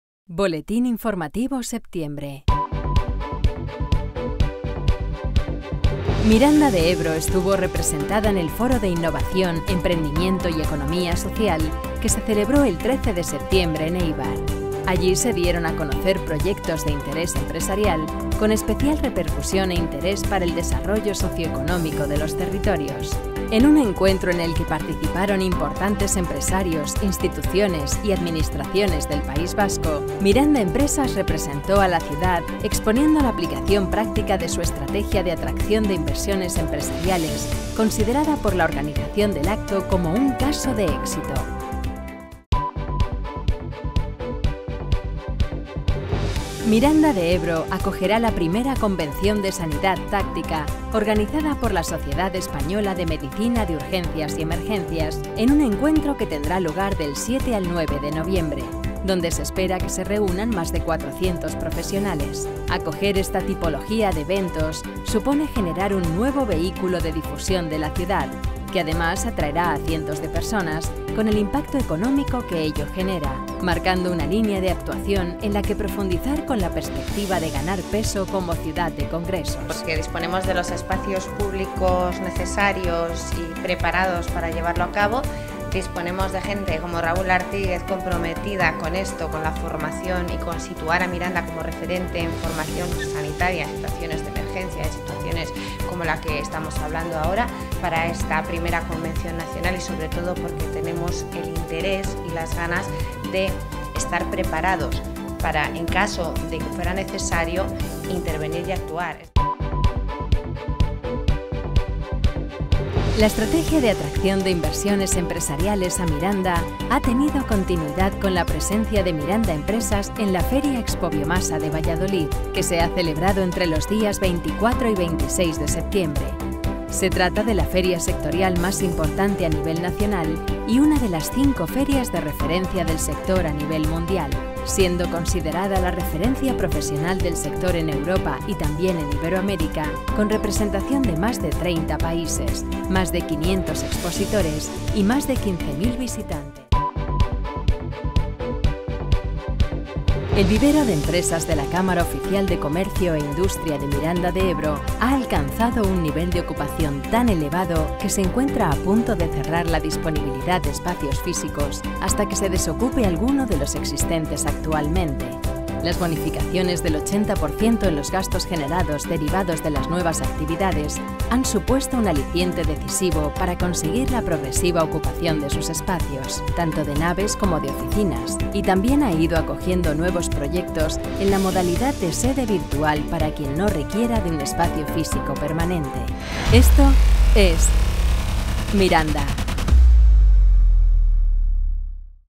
BOLETÍN INFORMATIVO · SEPTIEMBRE 2019
FORO DE INNOVACIÓN, EMPRENDIMIENTO Y ECONOMÍA SOCIAL: LA ESTRATEGIA DE MIRANDA, CONSIDERADA COMO CASO DE ÉXITO EN EL PAÍS VASCO
Miranda de Ebro estuvo representada en el Foro de Innovación, Emprendimiento y Economía Social que se celebró el 13 de septiembre en Eibar. Allí se dieron a conocer proyectos de interés empresarial, con especial repercusión e interés para el desarrollo socioeconómico de los territorios.

En un encuentro en el que participaron importantes empresarios, instituciones y administraciones del País Vasco, Miranda Empresas representó a la ciudad exponiendo la aplicación práctica de su estrategia de atracción de inversiones empresariales, considerada por la organización del acto como un caso de éxito.

I CONVENCIÓN DE SANIDAD TÁCTICA: MIRANDA ACOGERÁ UNA CONVENCIÓN DE REFERENCIA A NIVEL NACIONAL

Miranda de Ebro acogerá la I Convención de Sanidad Táctica, organizada por la Sociedad Española de Medicina de Urgencias y Emergencias, en un encuentro que tendrá lugar del 7 al 9 de noviembre, donde se espera que se reúnan más de 400 profesionales.

Acoger esta tipología de eventos supone generar un nuevo vehículo de difusión de la ciudad que, además, atraerá a cientos de personas, con el impacto económico que ello genera, marcando una línea de actuación en la que profundizar con la perspectiva de ganar peso como ciudad de Congresos.

Miranda de Ebro será durante esos tres días un foco de interés a nivel nacional. Se trata de la primera edición organizada en España.

EXPOBIOMASA: MIRANDA EMPRESAS ACUDE POR SEGUNDA VEZ

La estrategia de atracción de inversiones empresariales a Miranda ha tenido continuidad con la presencia de Miranda Empresas en La Feria Expobiomasa de Valladolid, que se ha celebrado entre los días 24 y 26 de septiembre.

Se trata de la Feria sectorial más importante a nivel nacional y una de las cinco ferias de referencia del sector a nivel mundial, siendo considerada la referencia profesional del sector en Europa y también en Iberoamérica, con representación de más de 30 países, más de 500 expositores, y más de 15.000 visitantes.

Es la vigesimoctava feria a la que se acude desde mediados de 2016.

VIVERO DE EMPRESAS: EL VIVERO ALCANZA UN NIVEL DE OCUPACIÓN MÁXIMO

El Vivero de Empresas de la Cámara Oficial de Comercio e Industria de Miranda de Ebro ha alcanzado un nivel de ocupación tan elevado que se encuentra a punto de cerrar la disponibilidad de espacios físicos hasta que se desocupe alguno de los existentes actualmente.

Las bonificaciones del 80% en los gastos generados derivados de las nuevas actividades han supuesto un aliciente decisivo para conseguir la progresiva ocupación de sus espacios, tanto de naves como de oficinas, y también ha ido acogiendo nuevos proyectos en la modalidad de sede virtual para quien no requiera de un espacio físico permanente.

De las existentes actualmente, sólo una actividad operaba hace 3 años. Tras ella, 15 nuevas actividades han entrado en el Vivero.